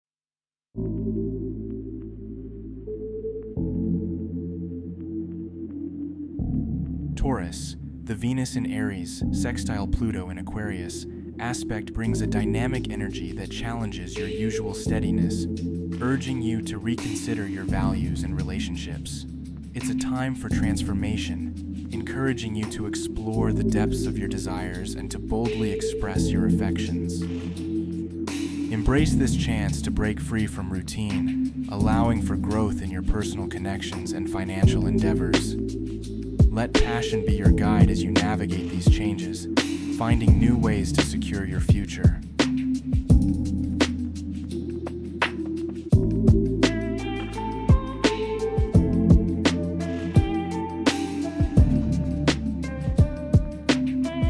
Daily Taurus Horoscope for April 6, 202
Taurus, the Venus in Aries sextile Pluto in Aquarius aspect brings a dynamic energy that challenges your usual steadiness, urging you to reconsider your values and relationships. It's a time for transformation, encouraging you to explore the depths of your desires and to boldly express your affections. Embrace this chance to break free from routine, allowing for growth in your personal connections and financial endeavors. Let passion be your guide as you navigate these changes, finding new ways to secure your future.